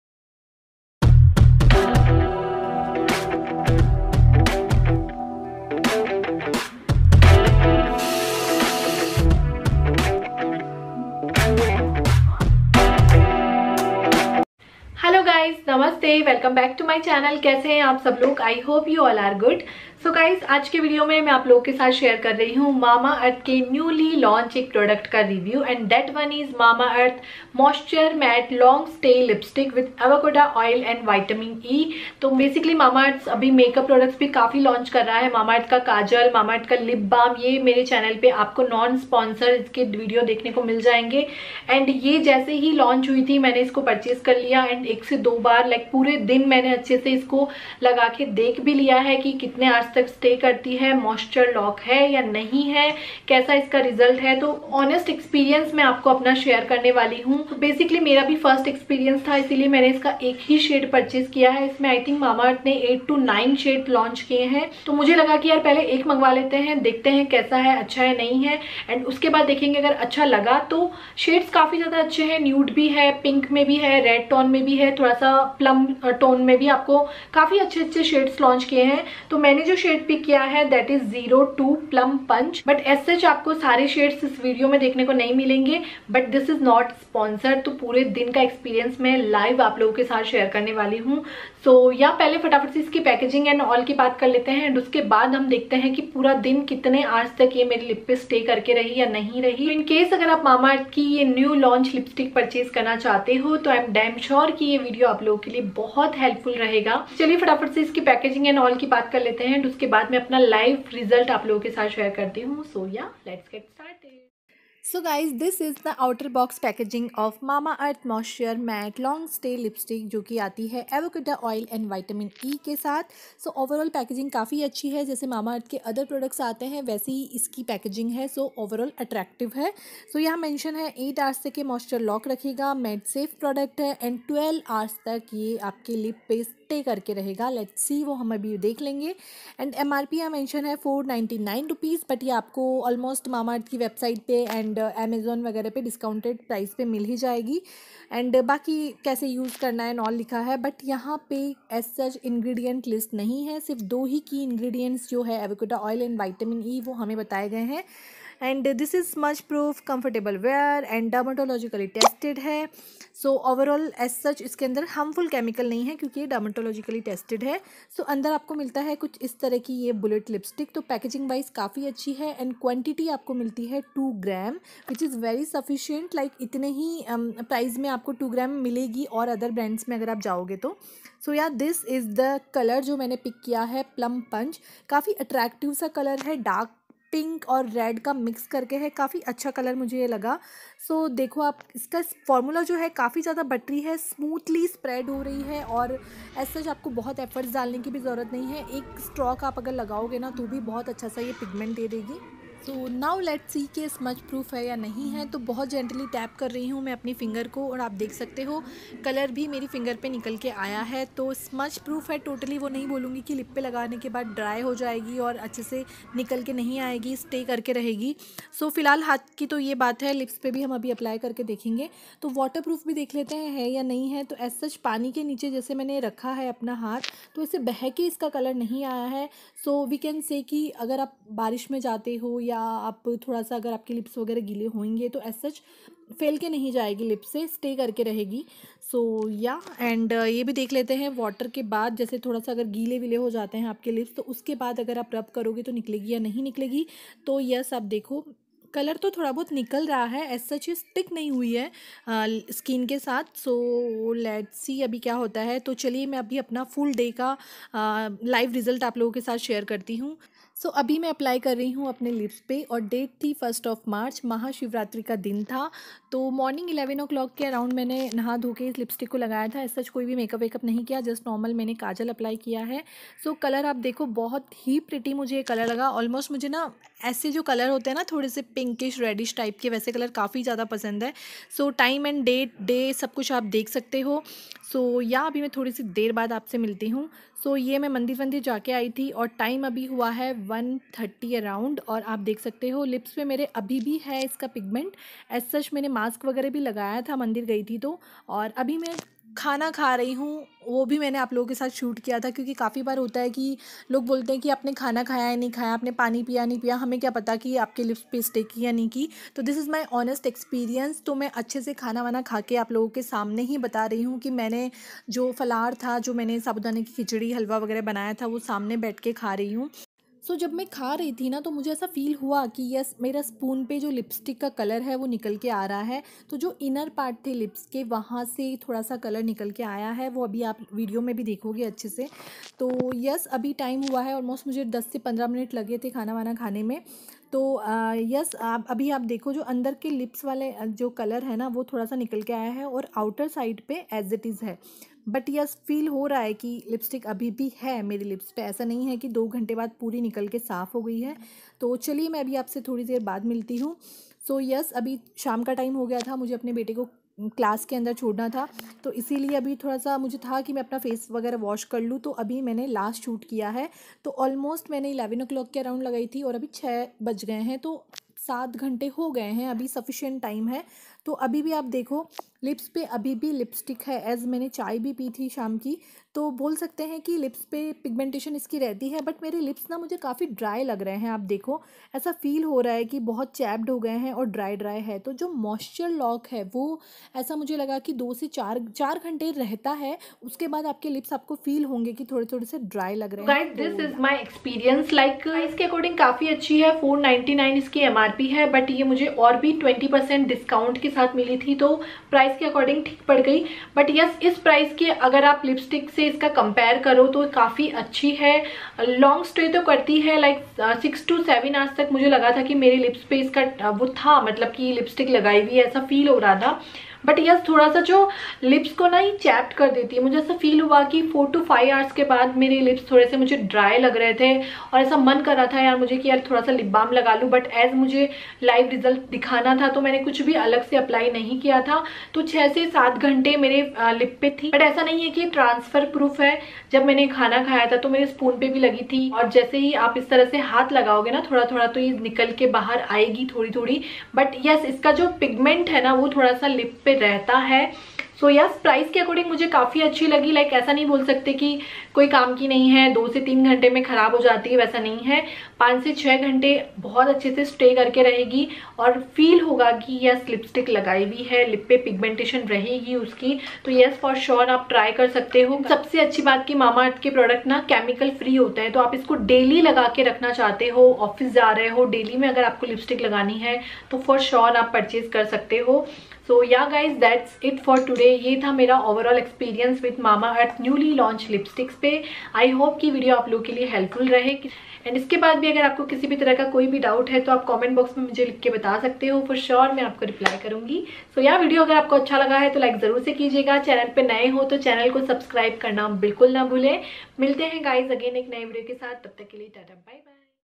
Mamaearth Lipstick Review | Live result, Non Sponsored | Mamaearth Moisture matte Long stay Lipstick
Hello Everyone!
Today I shared mamaearth longstay lipstick review with live result..this is jon Sponsored video n i am sharing full day live result..

 I hope my review helps you all understand the product better. And you all know what to expect from this product , if you decide to get it.